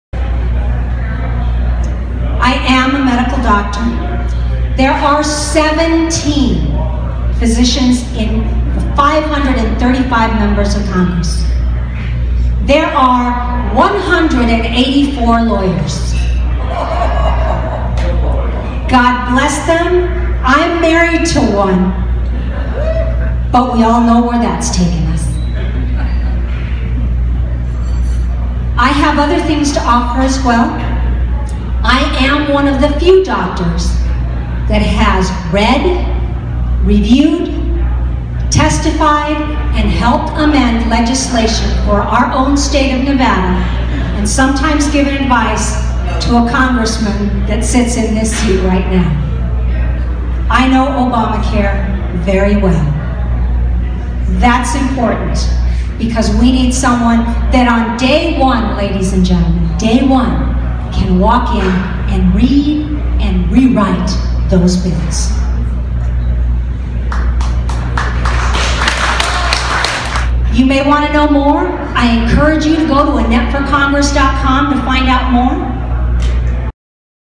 Vote Dr Annette Teijeiro NV CD3 - Opponents Fiore, Roberson, Matthews, Tarkanian
Vote for Dr. Annette Teijeiro for United States Congress Nevada Congressional District 3.

Her opponents are Danny Tarkanian, Andy Matthews, Michael Roberson and Michele Fiore.

Keep a doctor in NV CD3.